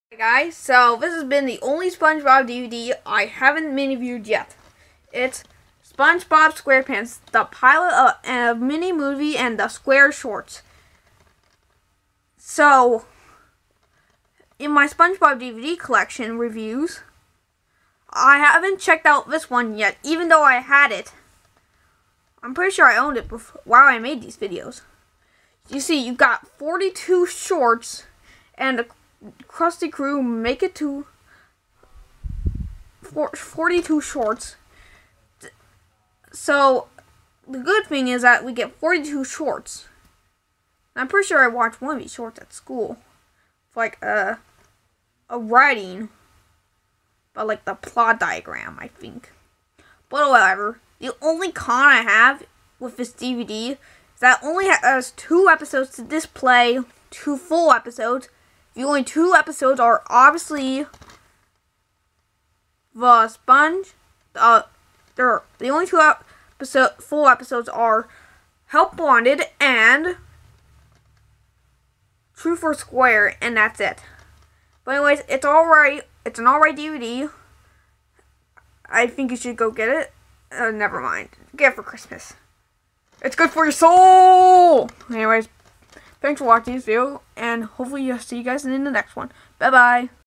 SpongeBob SquarePants The Pilot a Mini-Movie and the Square Shorts Miniview
So since I didn't review this DVD in my SpongeBob DVD Collection Videos I Decided to make this just so I can give out my Thoughts on this DVD find out what my opinion on this DVD is